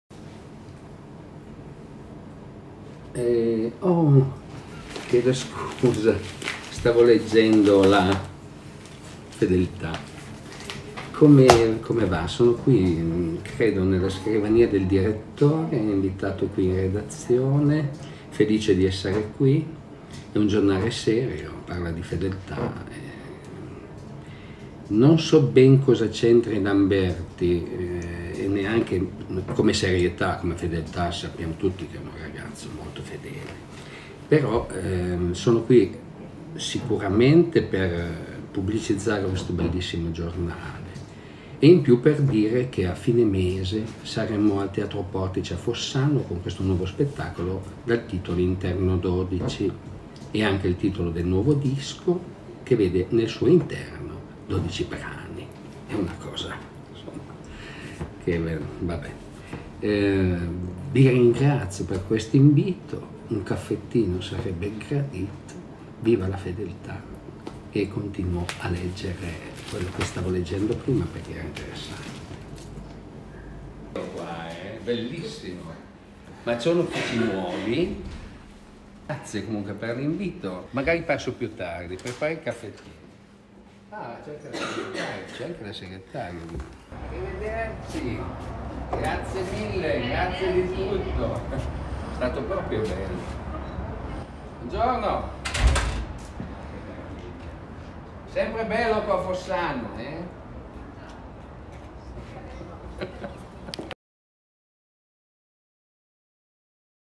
Padre Filip in visita a La Fedeltà
Manca poco al debutto fossanese del nuovo spettacolo di Pippo Bessone, in arte Padre Filip, e tutta il suo seguito fatto di artisti, musicisti e professionisti della risata. Interno 12 andrà in scena venerdì 31 gennaio 2020 alle 21 sul palco de I Portici ed è ormai prossimo il sold out (per saperne di più contattare direttaemente il teatro, tel. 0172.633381 oppure visitare il sito